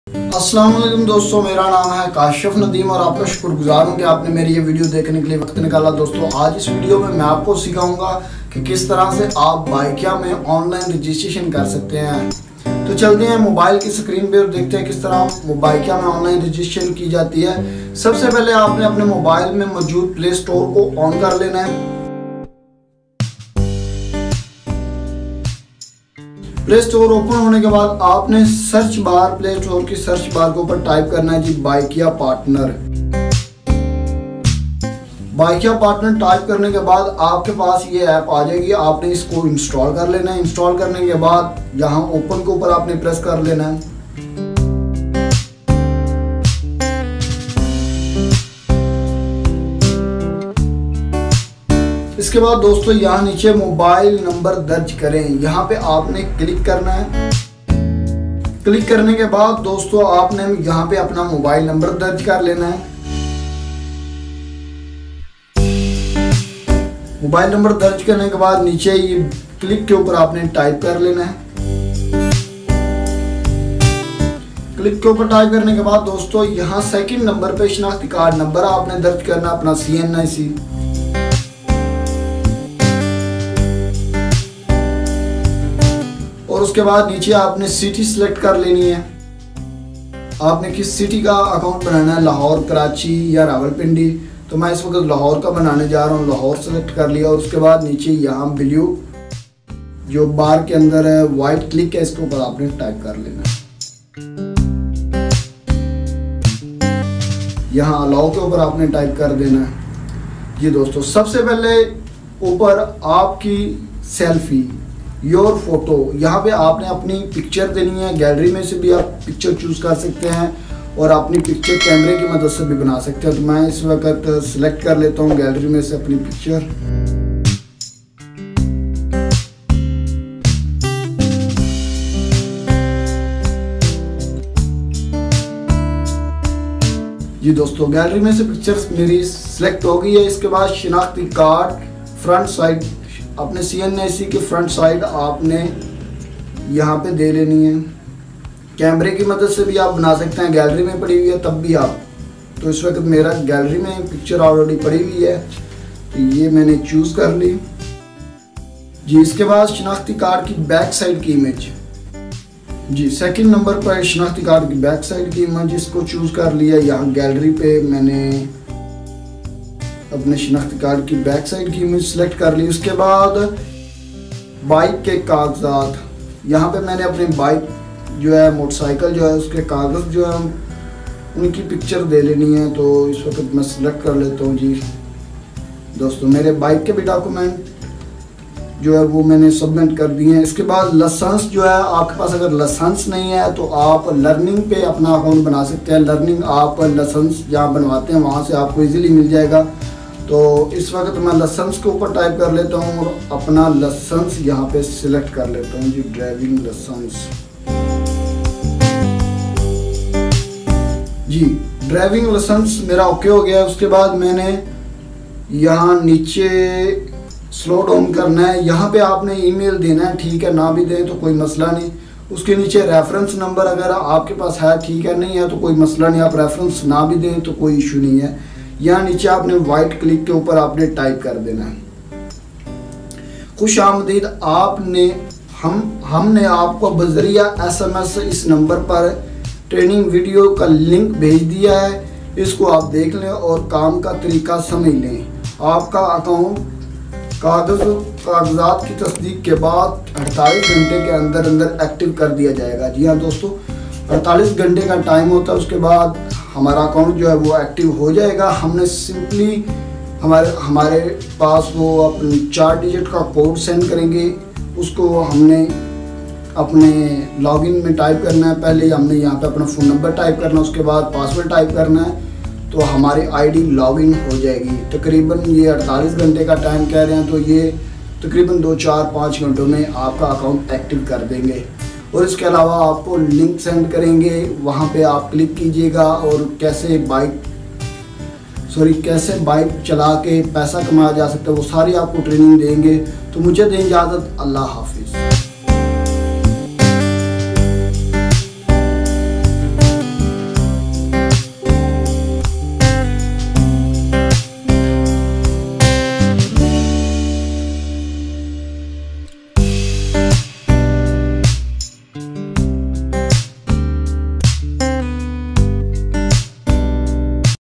Bykea Partner Online Registration 2021 | Bykea Partner id banane ka tarika | OK Mr
In This Video You Will Learn How to use bykea partner app.

Document;
1. Your Photo
2. ID Card
3. Driving Licence or Learner
4. Bike Registration Book
5. Phone NO

Bykea started its services in Karachi and now expanded to five more cities including Lahore, Multan, Rawalpindi, Peshawar, and Faisalabad.